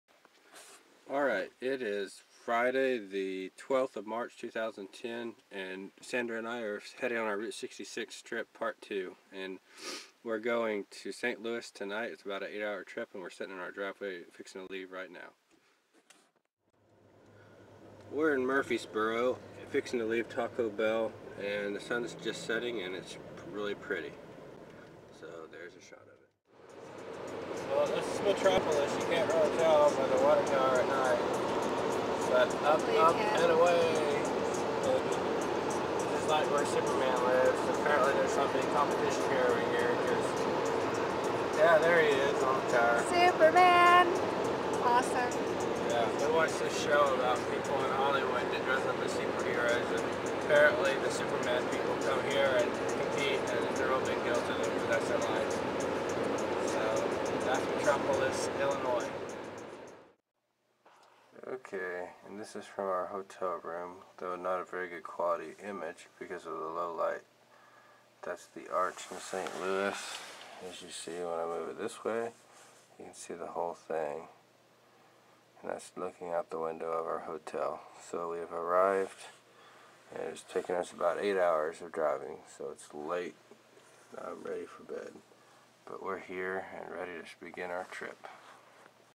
Friday 3-12-2010
Our trip to Saint Louis to begin part two of Route 66